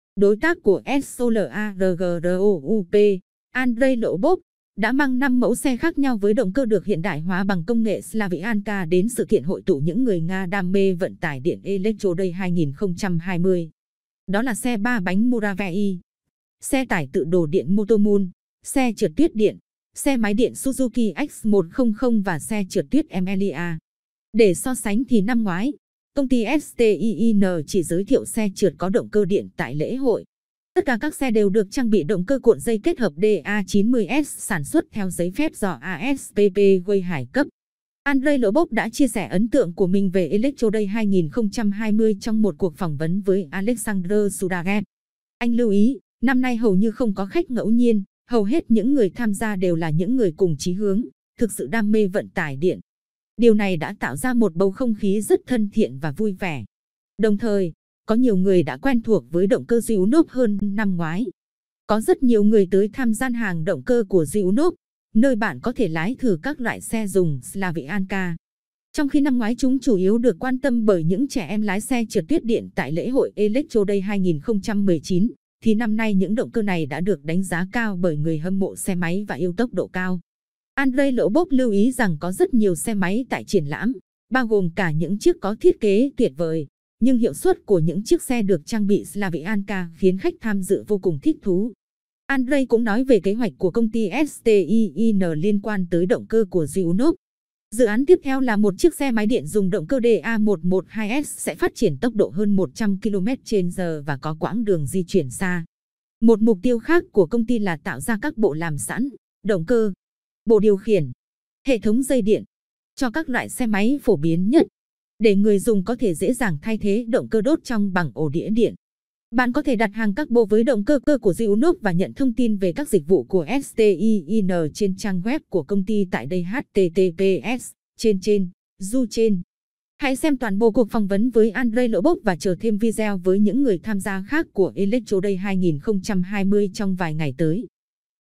đã nói về dự án tương lai với động cơ của Duyunov và sự kiện Electroday 2020 đã diễn ra như thế.
Đối tác của SOLARGROUP, Andrey Lobov, đã mang 5 mẫu xe khác nhau với động cơ được hiện đại hóa bằng công nghệ "Slavyanka" đến sự kiện hội tụ những người Nga đam mê vận tải điện Electroday 2020. Đó là xe ba bánh "Muravey" ("Ant"), xe tải tự đổ điện "Motomul", xe trượt tuyết điện, xe máy điện Suzuki AX-100 và "xe trượt tuyết Emelya". Để so sánh thì năm ngoái, công ty STIIN chỉ giới thiệu xe trượt có động cơ điện tại lễ hội. Tất cả các xe đều được trang bị động cơ cuộn dây kết hợp DA-90S sản xuất theo giấy phép do ASPP Weihai cấp.

Andrey Lobov đã chia sẻ ấn tượng của mình về Electroday 2020 trong một cuộc phỏng vấn với Alexander Sudarev. Anh lưu ý, năm nay hầu như không có khách ngẫu nhiên, hầu hết những người tham gia đều là những người cùng chí hướng, thực sự đam mê vận tải điện. Điều này đã tạo ra một bầu không khí rất thân thiện và vui vẻ. Đồng thời, có nhiều người đã quen thuộc với động cơ Duyunov hơn năm ngoái.

Có rất nhiều người tới tham gian hàng "Động cơ của Duyunov", nơi bạn có thể lái thử các loại xe dùng "Slavyanka". Trong khi năm ngoái chúng chủ yếu được quan tâm bởi những trẻ em lái xe trượt tuyết điện tại lễ hội Electroday 2019, thì năm nay những động cơ này đã được đánh giá cao bởi người hâm mộ xe máy và yêu tốc độ cao. Andrey Lobov lưu ý rằng có rất nhiều xe máy tại triển lãm, bao gồm cả những chiếc có thiết kế tuyệt vời, nhưng hiệu suất của những chiếc xe được trang bị "Slavyanka" khiến khách tham dự vô cùng thích thú.

Andrey cũng nói về kế hoạch của công ty STIIN liên quan tới động cơ của Duyunov. Dự án tiếp theo là một chiếc xe máy điện dùng động cơ DA-112S sẽ phát triển tốc độ hơn 100 km/h và có quãng đường di chuyển xa.

Một mục tiêu khác của công ty là tạo ra các bộ làm sẵn (động cơ, bộ điều khiển, hệ thống dây điện) cho các loại xe máy phổ biến nhất, để người dùng có thể dễ dàng thay thế động cơ đốt trong bằng ổ đĩa điện.

Bạn có thể đặt hàng các bộ với động cơ của Duyunov và nhận thông tin về các dịch vụ của STIIN trên trang web của công ty tại đây

Hãy xem toàn bộ cuộc phỏng vấn với Andrey Lobov và chờ thêm video với những người tham gia khác của Electroday 2020 trong vài ngày tới!